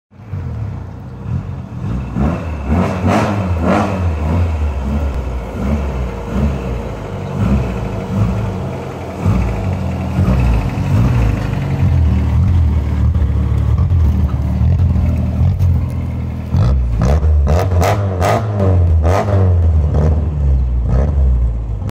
Stanced Escort XR3#escort#xr3#1986
Baby Blue Ford Escort on some modern wheels..